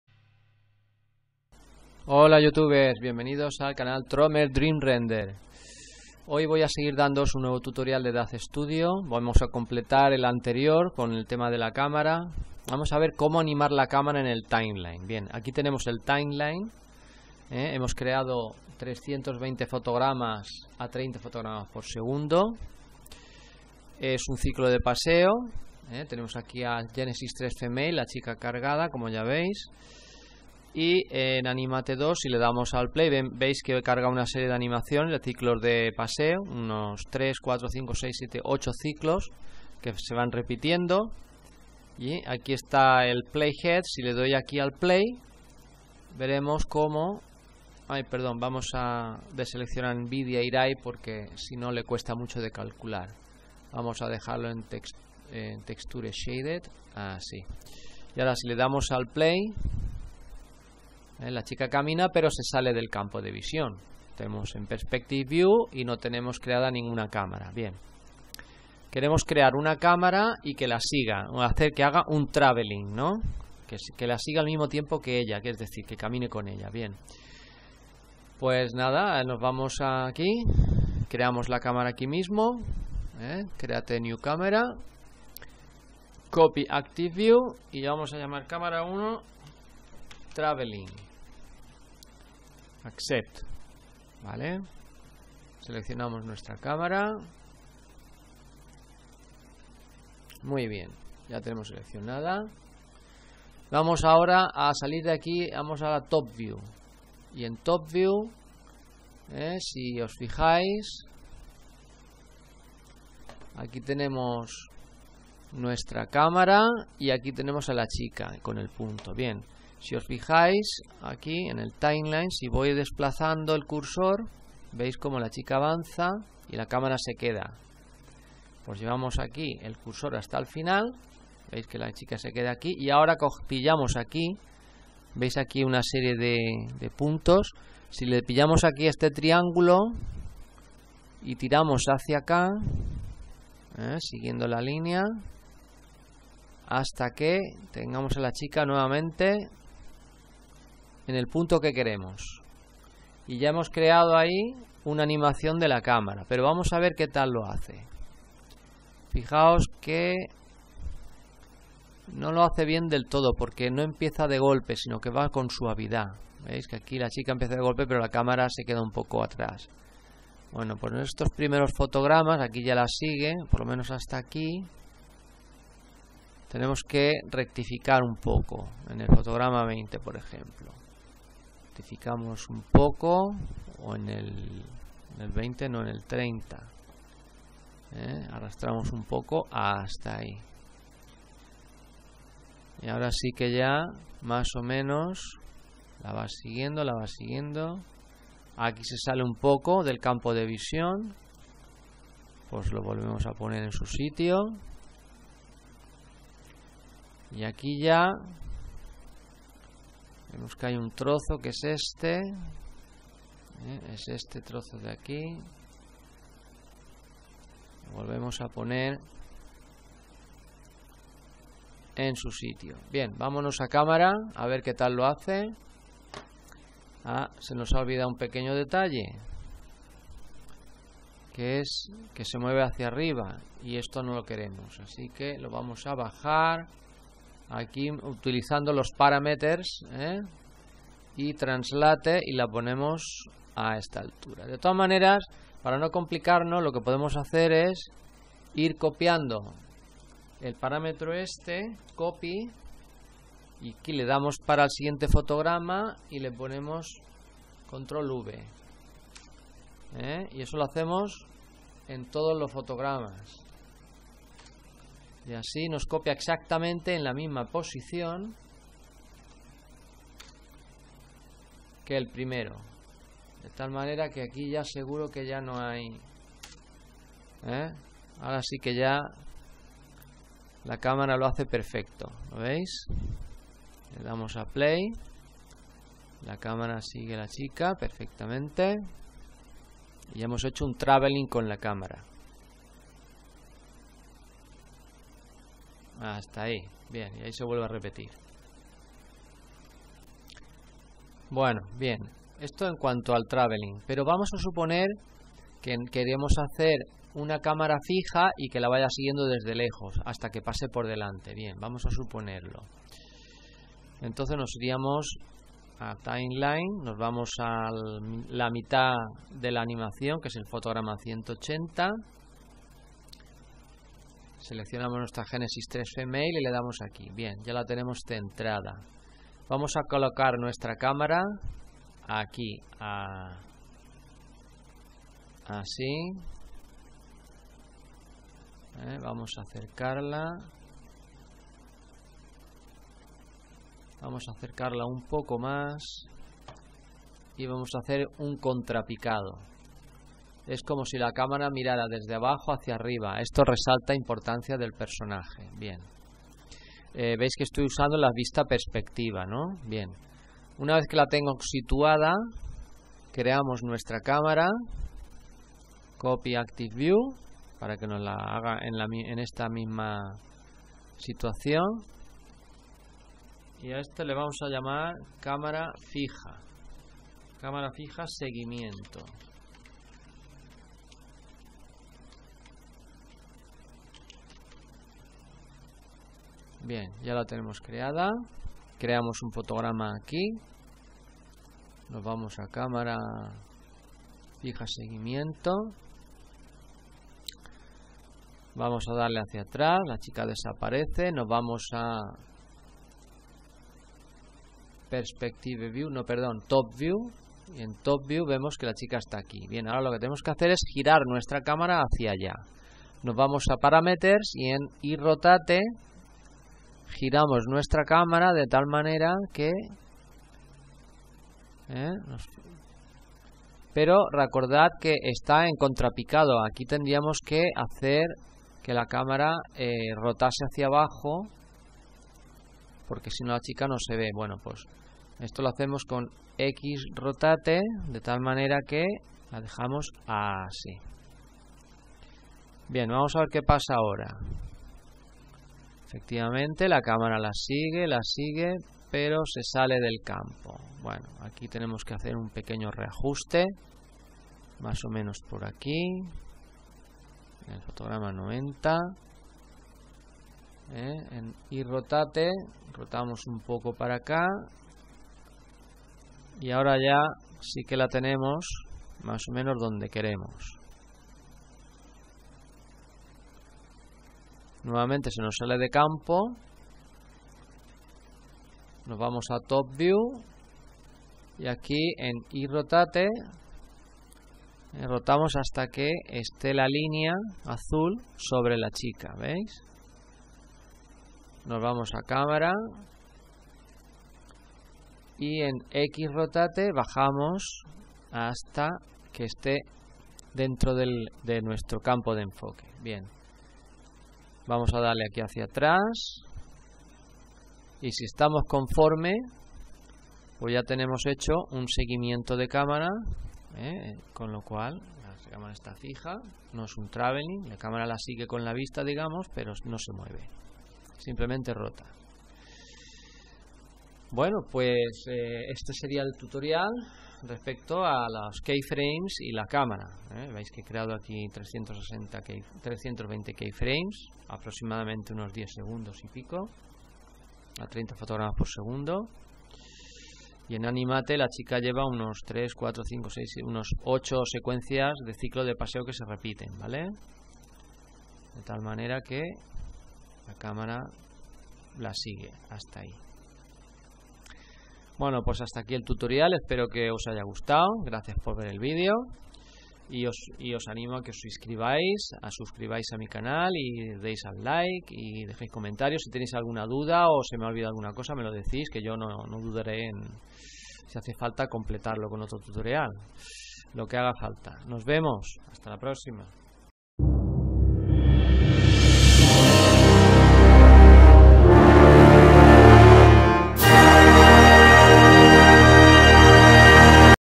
How To Create Keys in The Timeline to Animate The Camera! Daz Studio Tutorial in Spanish
How To Create Keys in The Timeline to Animate The Camera!  Daz Studio Tutorial
This is a daz studio tutorial video in espanol language.

Importance of Body Language in Communication

Thanks to Trommer Dream render Animation for this animation video daz studio tutorial.
Hope you enjoyed the video and found it helpful.
Watch the complete video and properly observe the demonstration.